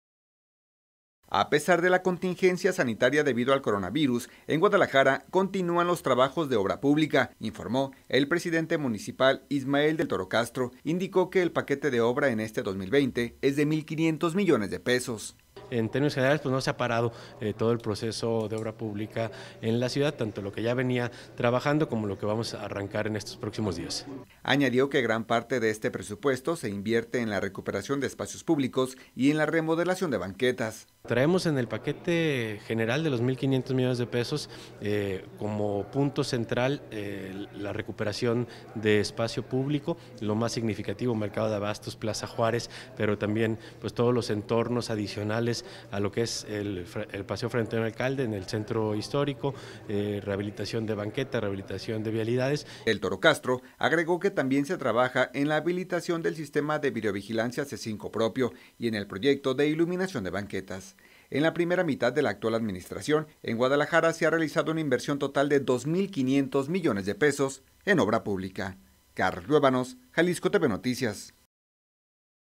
En Jalisco pretenden que la obra pública sea motor de reactivación | JaliscoTV
Canales: 17.1 tv abierta,  117 megacable, 17  izzi, 17  totalplay 17 y 154 axteltv,
17 cosmored.